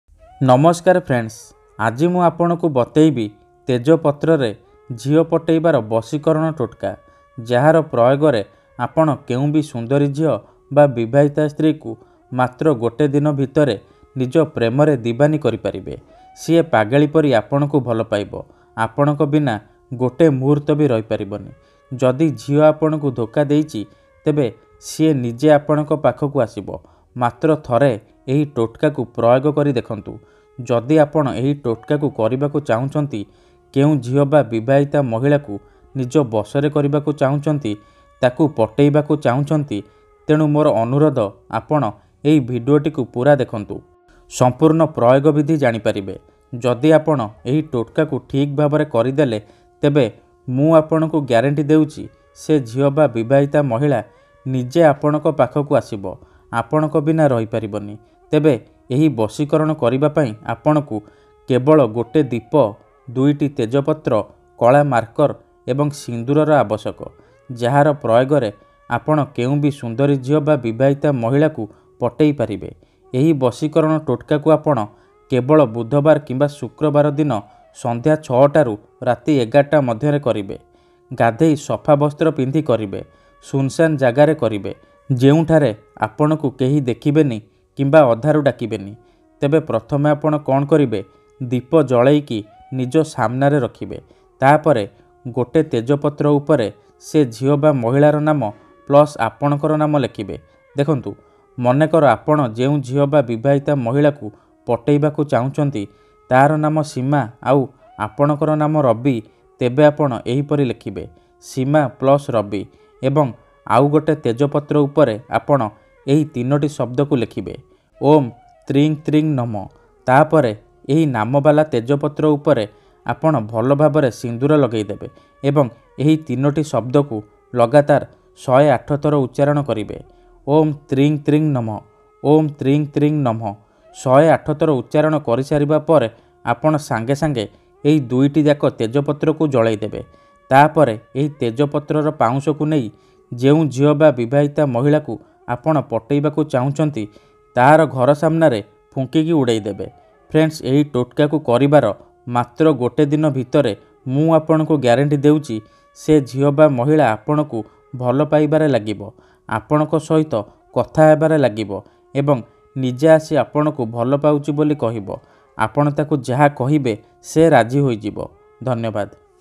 ତେଜ ପତ୍ରରେ ଲେଖି ଦିଅନ୍ତୁ ଏହି ଶବ୍ଦ ଝିଅ ପଟିଯିବ | odia vashikaran
ତେଜ ପତ୍ରରେ ଲେଖି ଦିଅନ୍ତୁ ଏହି ଶବ୍ଦ ଝିଅ ପଟିଯିବ | odia vashikaran | vashikaran | vashikaran mantra | vashikaran odia |

Namaskar friends ajira ehi video tire mu apanaku teja patra re emti eka saktisali vashikaran totka bisayare kahichi jahaku prayaga kari apana pasand karuthiba kounasi jhiaku ba mahilaku patei paribe